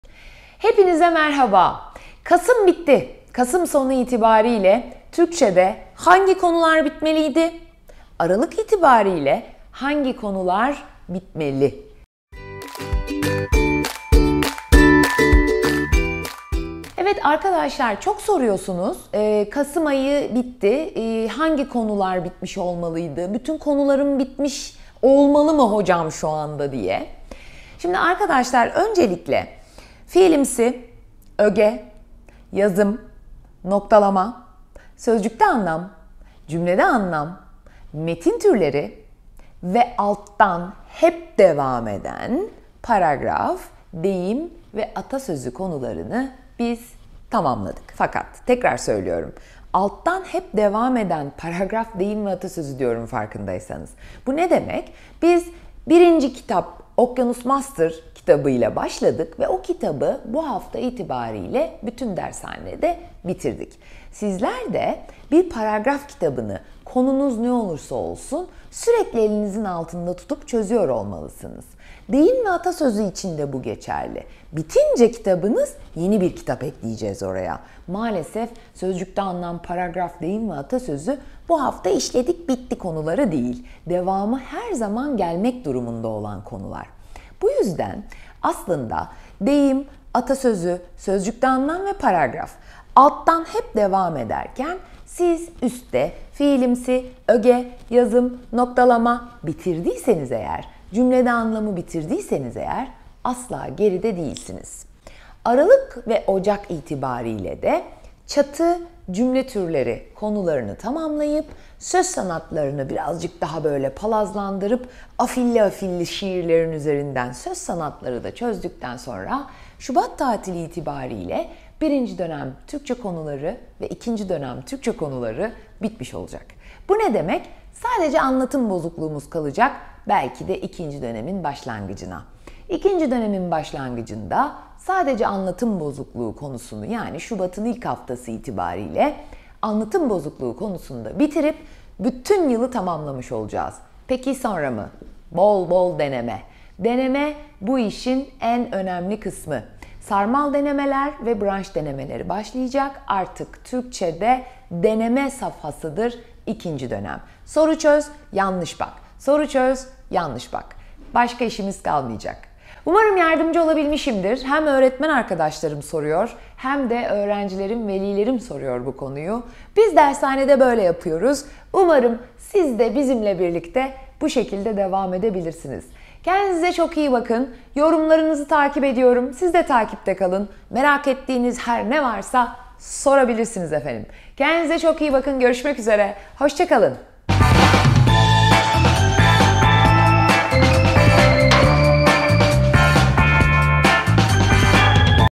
LGS öğrencileri kasım sonuna kadar ne yapmalıydı? Aralık ayında neler yapılmalı ?
LGS öğrencileri kasım sonuna kadar ne yapmalıydı? Aralık ayında yapılması gerekenler neler?

Kasım ayı kritik aylardan biridir. Zayıf öğrenci pes etmek ister, iyi öğrenci kaynak çoğaltmak ister.
Peki doğrusu ne?

Neslihan Çopur "şimdiye kadar neler yapılmalıydı, aralık ayı itibari ile ne yapılması gerekir?" sorularını yanıtlıyor.